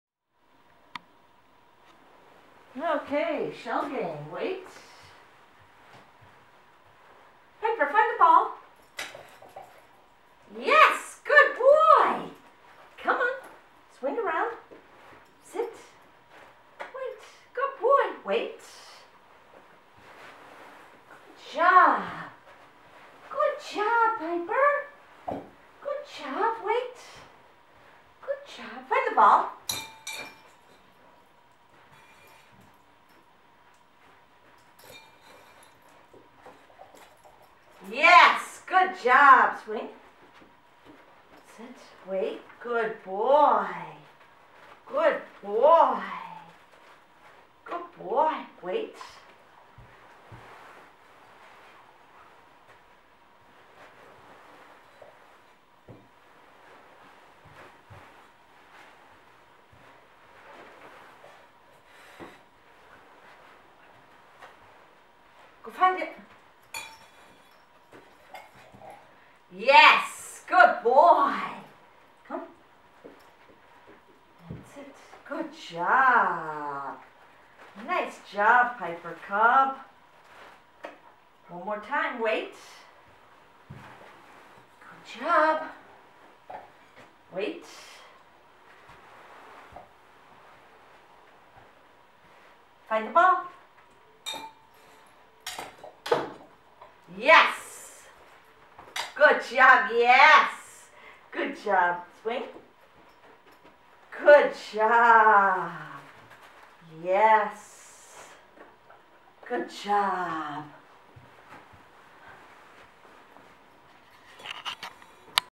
023
Piper Cub performs "Shell Game" to obtain Expert Certification Trick Title